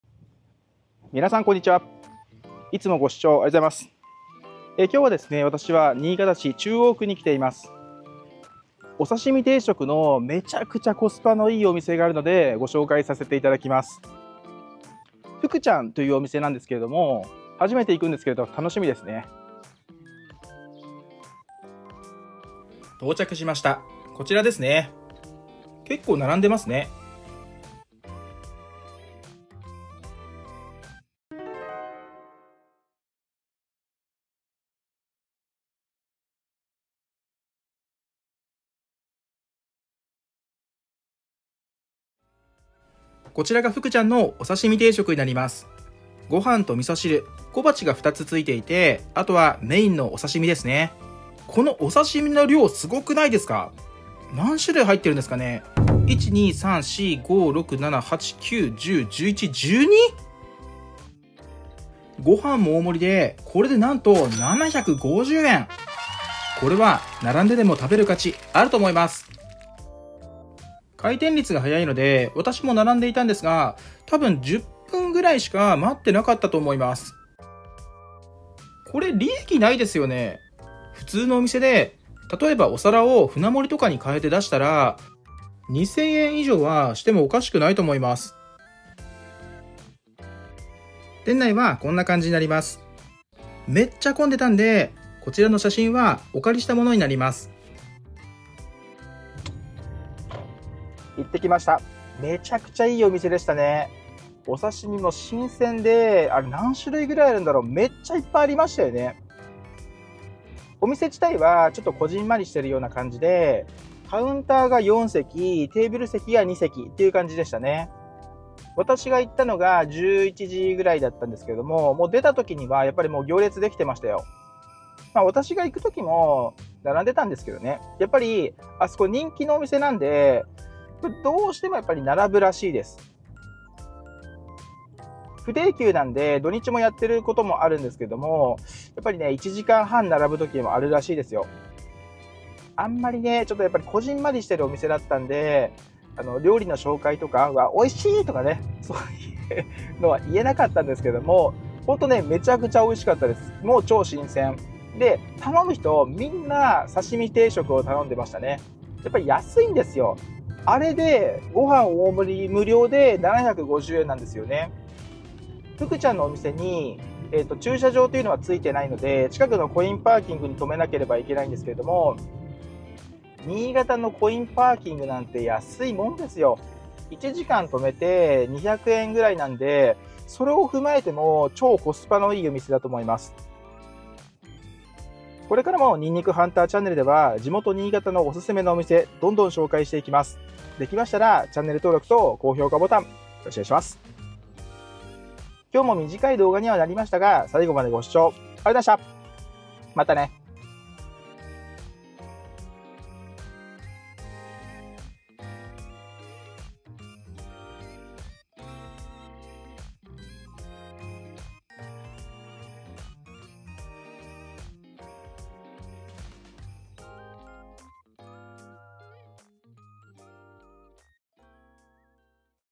【価格破壊】福ちゃん コスパ最強の刺身定食 これで750円 新潟
仕入れにより内容は変わると思いますが安過ぎてまた閉店しないか心配です福ちゃんのお刺身定食はボリューム満点で新鮮！

キャンプ動画おすすめ